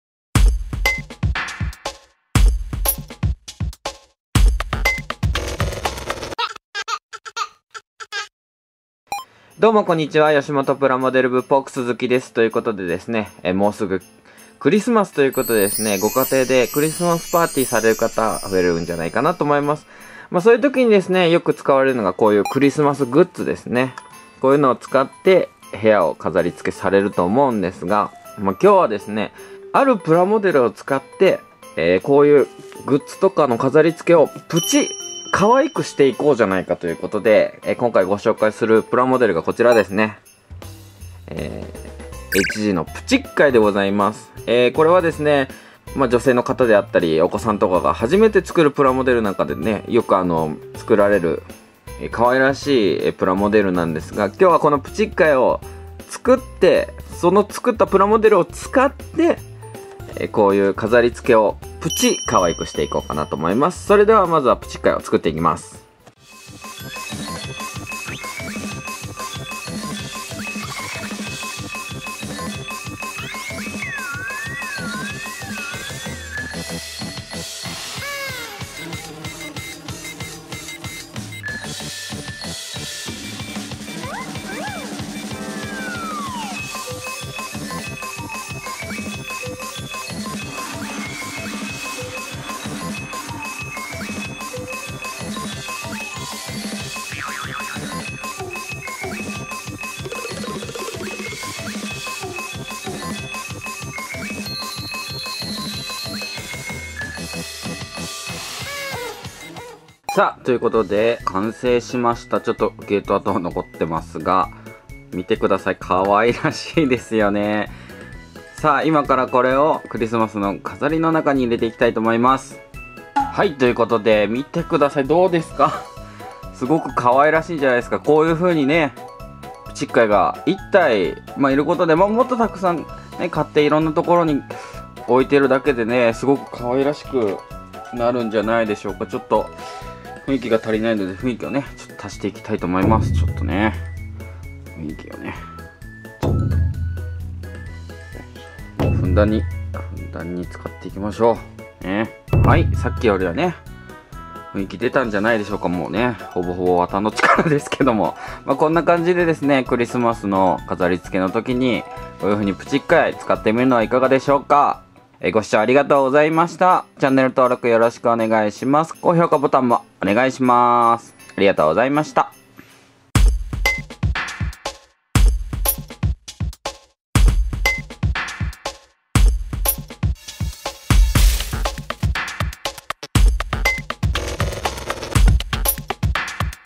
【X'mas】クリスマス飾りをプチッガイでプチかわいく!!
吉本プラモデル部チャンネルです!!

もうすぐクリスマス!!
クリスマス飾りをプチッガイでプチかわいくしてみました!!
クリスマスパーティーで喜ばれること間違い無し!!